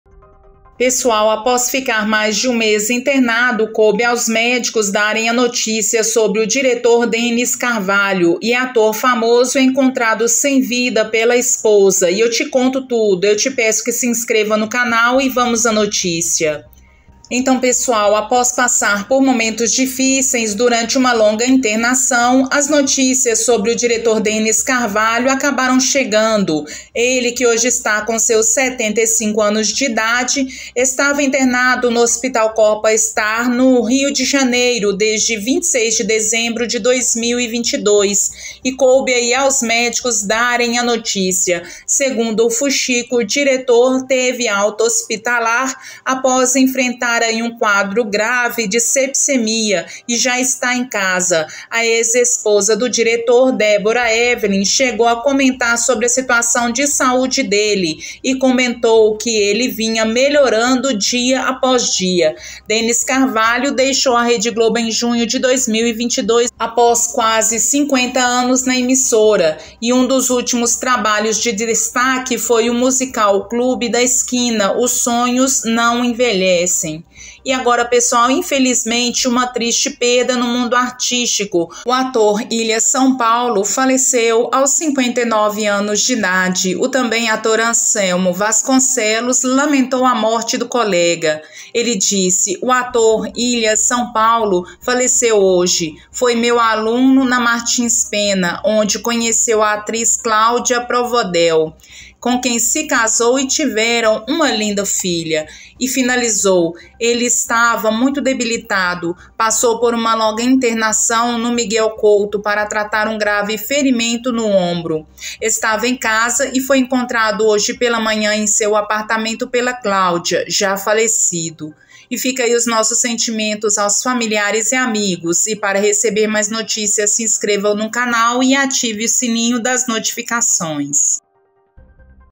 ATOR DA GLOBO ENCONTRADO PELA ESPOSA SEM VIDA... APÓS LONGA INTERNAÇÃO, DENNIS CARVALHO, AOS 75 ANOS
Pessoal, após ficar mais de 1 mês internado, coube aos médicos darem a notícia sobre o diretor Dennis Carvalho e ator famoso é encontrado sem vida pela esposa e eu te conto tudo.

Então pessoal, após passar por momentos difíceis, durante uma longa internação, as notícias sobre o diretor Dennis Carvalho acabaram chegando. Ele, hoje com 75 anos de idade, estava internado no hospital Copa Star no Rio de Janeiro, desde 26 de dezembro de 2022, coube ao médico dar a notícia.

Segundo O fuxico, o diretor teve alta hospitalar após enfrentar um quadro de grave septicemia e já está em casa. A ex-esposa do diretor, Deborah Evelyn, chegou a comentar sobre a situação da saúde dele. E comentou que ele vinha melhorando dia após dia.

Dennis Carvalho deixou a rede Globo em junho de 2022 após quase 50 anos na emissora. E um dos últimos trabalhos de destaque foi o musical " Clube da Esquina – Os sonhos não envelhecem”.

E agora pessoal, infelizmente uma triste perda no mundo artístico. O ator Ilya São Paulo faleceu aos 59 anos. O ator Anselmo Vasconcellos, lamentou a morte do colega:

“O ator Ilya São Paulo faleceu hoje. Foi meu aluno na Martins Penna onde conheceu a atriz Cláudia Provedel, com quem casou e tiveram uma linda filha." E finalizou: "ele estava muito debilitado.

Passou por uma longa internação no Miguel Couto para tratar um grave ferimento no ombro. Estava em casa e foi encontrado hoje pela manhã em seu apartamento pela Claudia, já falecido”. Fica os nossos sentimentos aos familiares e amigos.